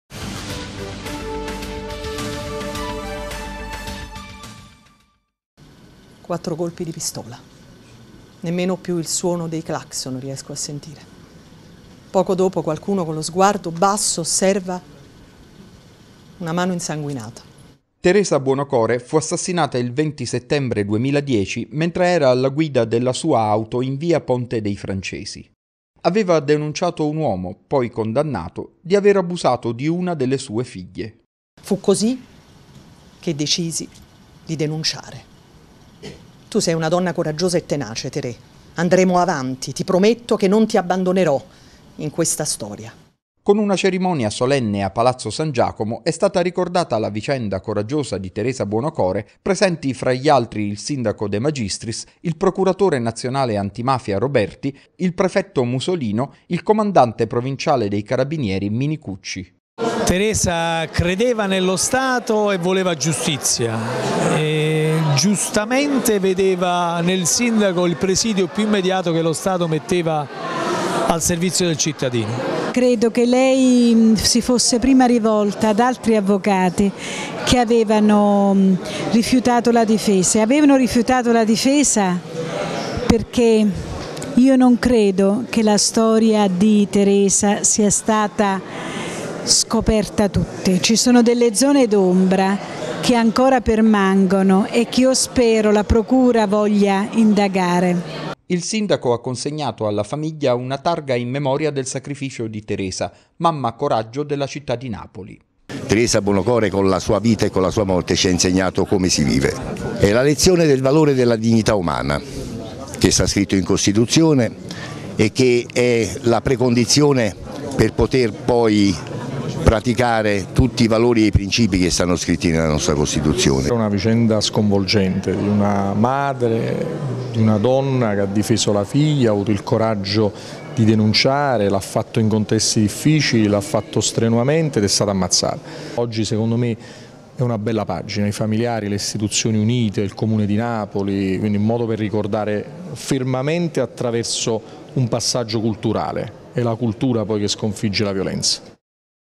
In memoria di Teresa Buonocore
Il sindaco consegna una targa ai familiari di Teresa Buonocore "mamma coraggio della Città di Napoili", in memoria del suo coraggioso gesto di denuncia.